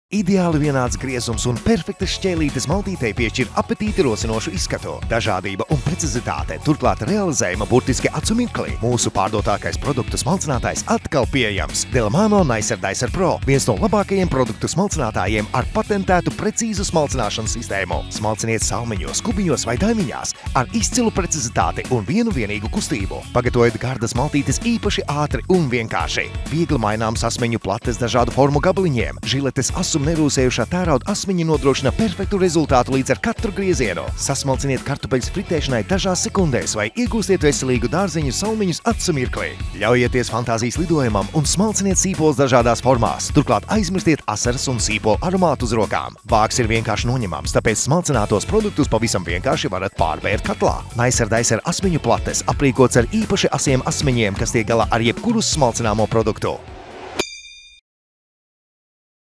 Produktu smalcinātājs Brava Nicer Dicer Pro
Pateicoties Delimano Produktu smalcinātājam Brava Nicer Dicer Pro, ēdiena gatavošana noritēs daudz ātrāk un ērtāk. Neslīdošā kājiņa nodrošina stabilitāti, bet 3 nomaināmie asmeņi ļaus sagriezt produktus 5 dažādos lielumos. Ļoti asie, nerūsējošā tērauda asmeņi gādās, lai jūs bez piepūles iegūtu vienmērīgi sagrieztas ēdiena sastāvdaļas. Trauka ietilpība: 800 ml. Viegli tīrāms un uzglabājams.